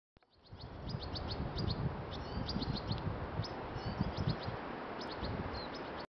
European Goldfinch (in NZ) cheeps (bird call/sound)
Jan 11, 2020 Wellington Botanical Gardens, Wellington, New Zealand
Female European Goldfinch (left) and male (on right with red on head that flies out of frame) cheep back and forth